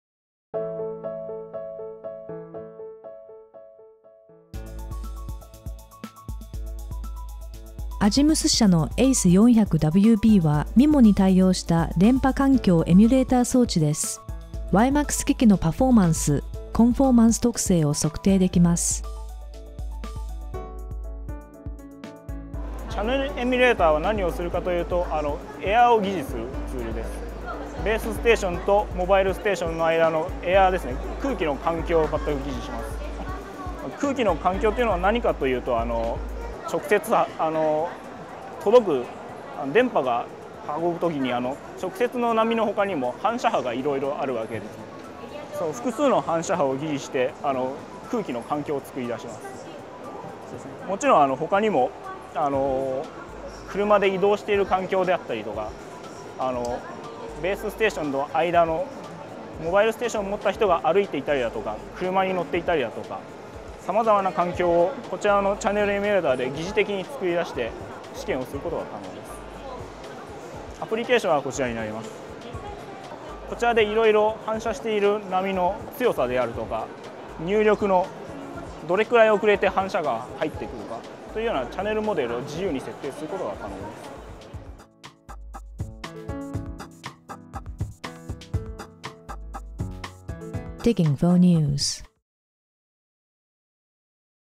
米Azimuth社 MIMO対応 WiMAX チャネルエミュレータ : DigInfo
Azimuth社の「ACE 400WB」は、MIMOに対応した電波環境エミュレータ装置です。WiMAX機器のパフォーマンス、コンフォーマンス特性を測定できます。

Interop Tokyo 2008